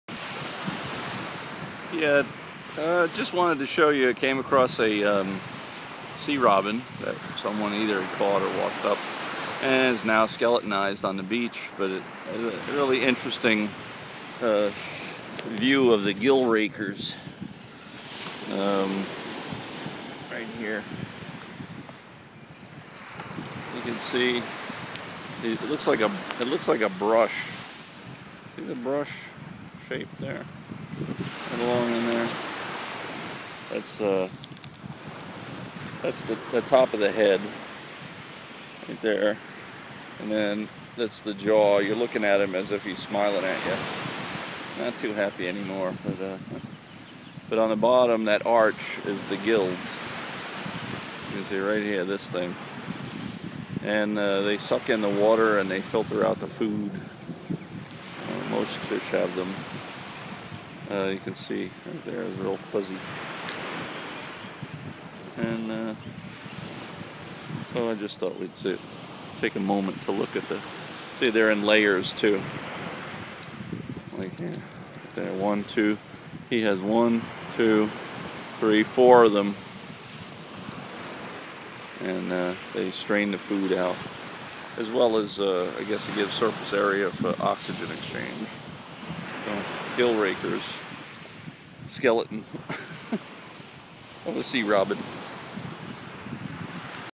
Fish gill raker
Sea robin skeleton showing gill raker